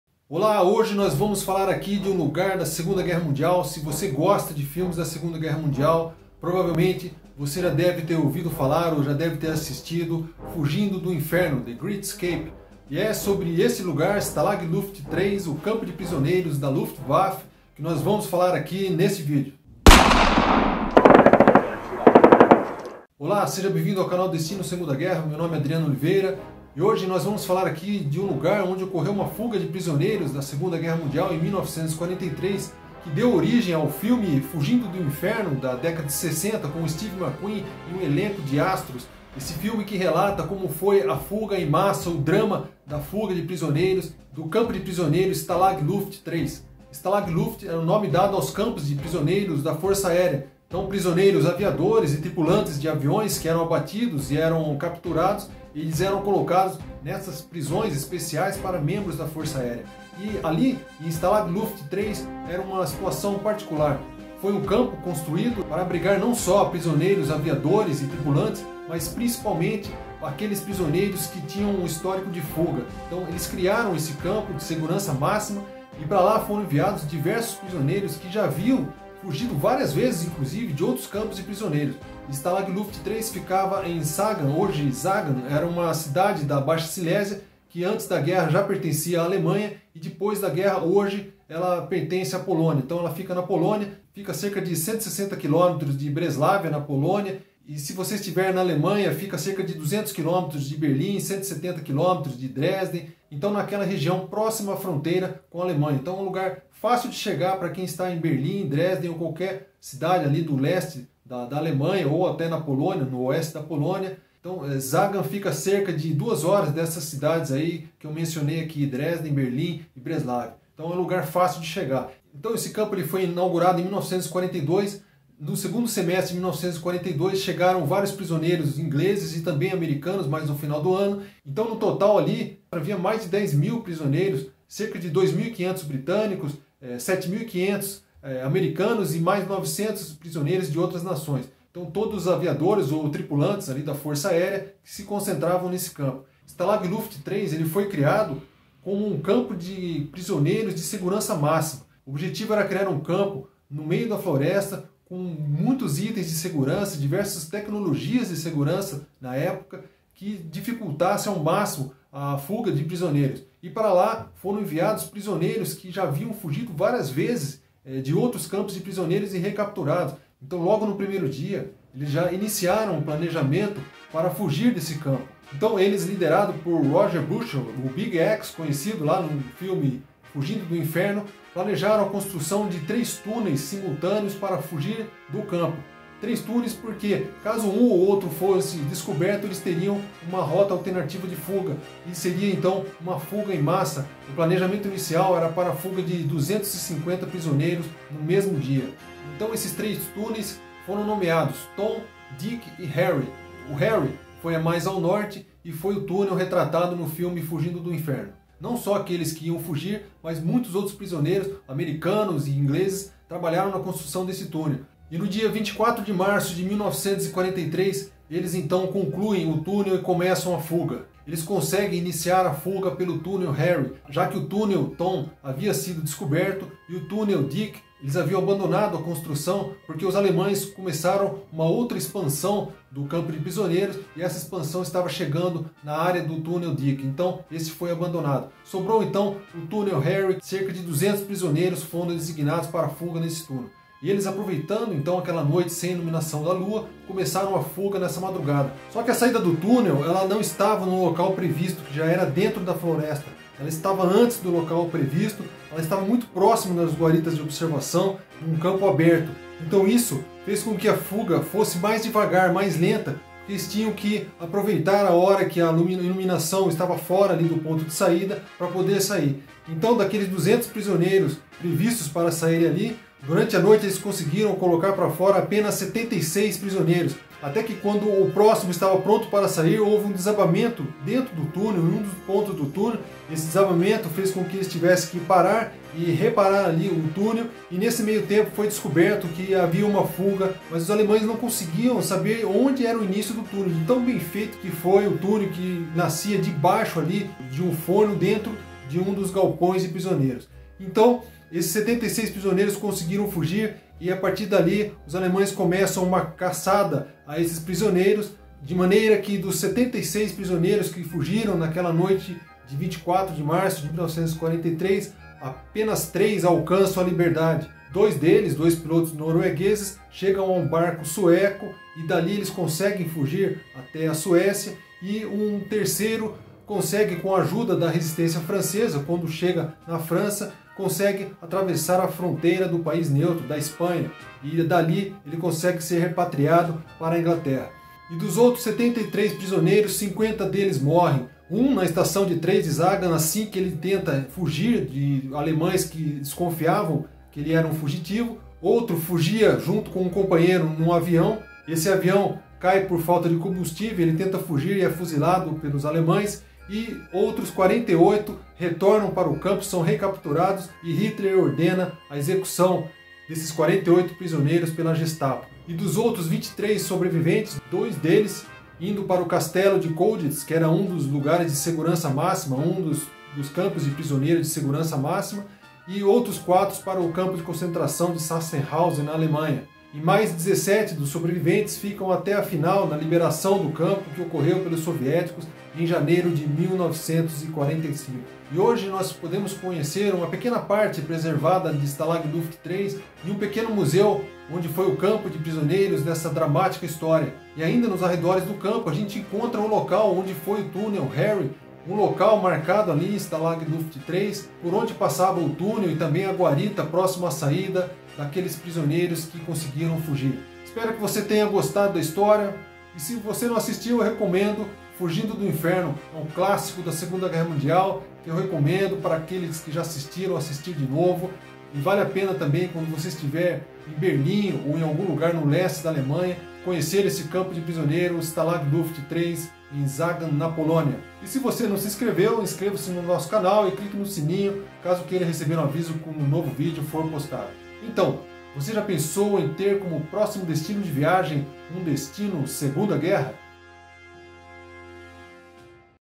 Fugindo do Inferno - Onde fica Stalag Luft III, que deu origem ao clássico da Segunda Guerra Mundial
Uma visita e uma breve história sobre o campo de prisioneiros Stalag Luft III, que foi palco da maior fuga em massa de prisioneiros da Segunda Guerra Mundial, e deu origem ao filme Fugindo do Inferno, The Great Scape (1963), um clássico do cinema, com Steve McQueen, James Garner, James Coburn, Charles Bronson, Richard Attemborough, Donald Pleasence, dentre outros.

CRÉDITOS
Fotos: 0:15 - "Stalag Luft Prison Camp" by rocbolt is licensed under CC BY-NC 2.0; 2:30 - Wikigraphists of the Graphic Lab (fr) CC BY-SA 3.0 , via Wikimedia Commons; 3:25 - nl:Dagoos at nl.wikipedia, CC BY-SA 2.5, via Wikimedia Commons; 5:42 - "Żagań, Poland. Parowóz Ol49 90 with train to Lubsko. November 1989" by sludgegulper is licensed under CC BY-SA 2.0; 6:29 - "File:Sagan camp-4.jpg" by vorwerk is licensed under CC BY-SA 3.0; 6:33 / 6:42 / 6:45 / 6:48 - vorwerk, CC BY-SA 3.0 , via Wikimedia Commons; 6:36 - Jacques Lahitte, CC BY 3.0 , via Wikimedia Commons.